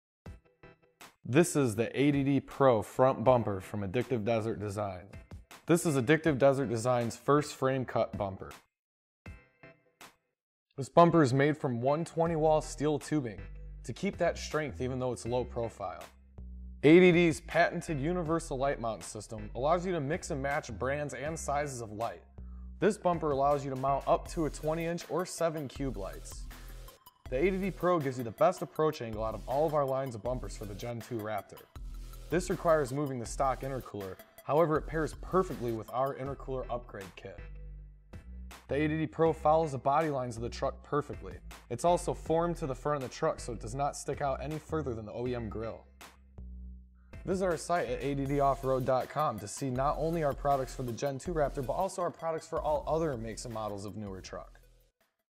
Ford Raptor ADD PRO Frame Cut Front Bumper
Combining aesthetics with functionality, the ADD PRO frame cut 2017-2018 Ford Raptor front bumper doesn't sacrifice the look of your truck for approach angle. Our ADD PRO bumper is the first true high quality frame cut bumper for the Ford Raptor giving your truck maximum approach angle for your off-road adventures.

The ADD PRO front bumper truly contours the body lines of the Raptor without the need of a plastic valence. Built with .120" wall steel tubing for strength and durability and an aluminum skid plate for protection. Mounting options for lights underneath the main tube to keep them tucked away allow for up to 7 cube lights or a 20" radius LED bar.